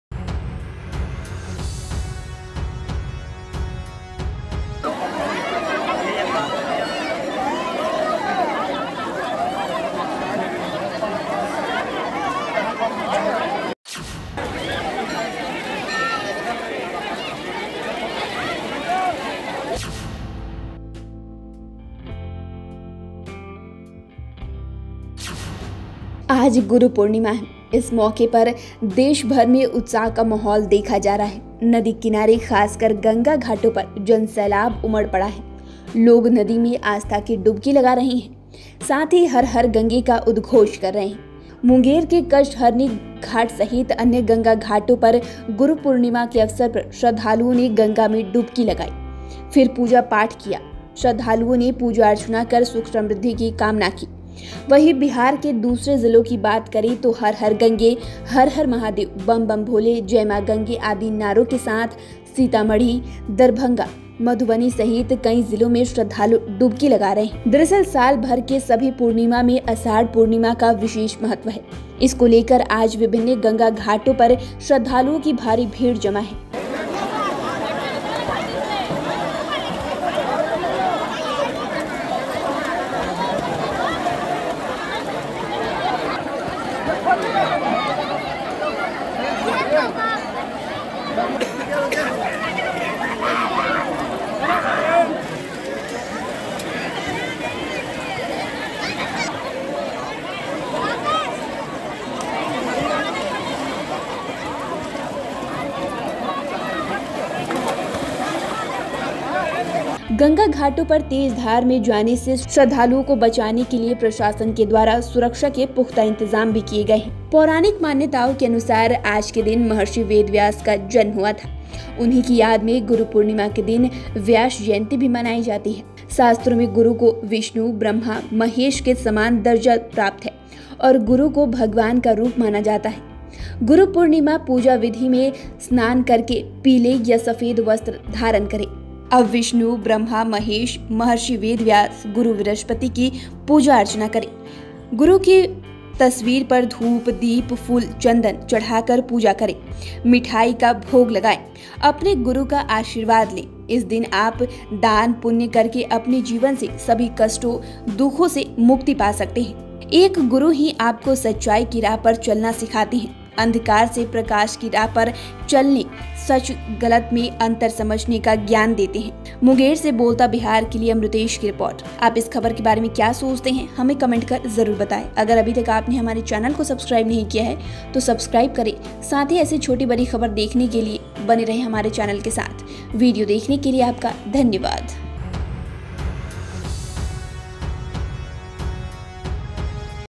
Guru Purnima पर Munger हुआ भगवामय, गंगा घटों पर श्रद्धालुओं ने लगाई आस्था की डुबकी | Bolta Bihar
देखें वीडियो...मुंगेर में गंगा स्नान के लिए उमड़ी भीड़, हर हर गंगे के उद्घोष से गूंजा घाट